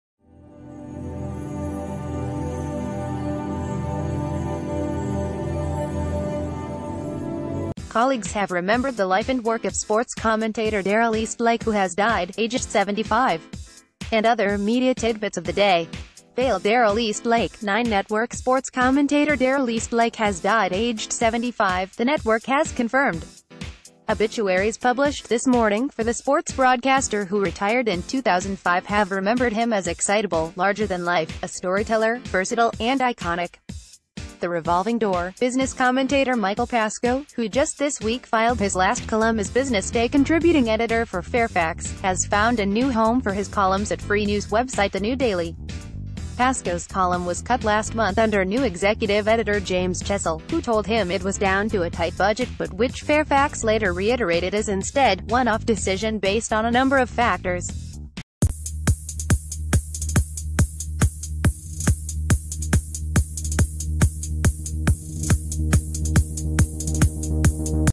Breaking News | Nine Network sports commentator Darrell Eastlake remembered
Colleagues have remembered the life and work of sports commentator Darrell Eastlake who has died, aged 75. And other media tidbits of the day. Vale Darrell Eastlake. Nine Network sports commentator Darrell Eastlake has died aged 75, the network has confirmed. Obituaries published this morning for the sports broadcaster who retired in 2005 have remembered him as excitable, larger-than-life, a storyteller, versatile, and iconic. The revolving door. Business commentator Michael Pascoe, who just thi...